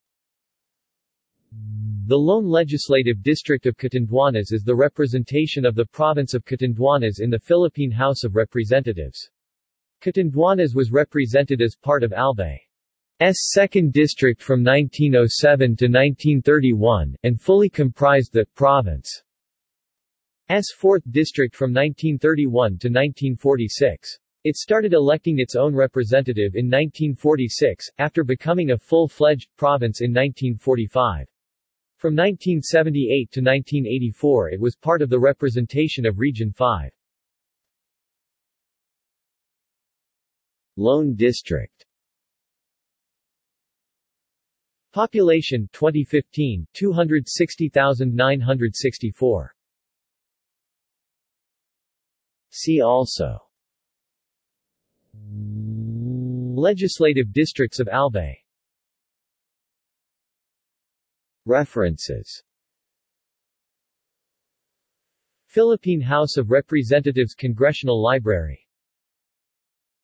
Legislative district of Catanduanes | Wikipedia audio article
This is an audio version of the Wikipedia Article:
Legislative district of Catanduanes

SUMMARY
The lone Legislative District of Catanduanes is the representation of the Province of Catanduanes in the Philippine House of Representatives. Catanduanes was represented as part of Albay's second district from 1907 to 1931, and fully comprised that province's fourth district from 1931 to 1946. It started electing its own representative in 1946, after becoming a full-fledged province in 1945. From 1978 to 1984 it was part of the representation of Region V.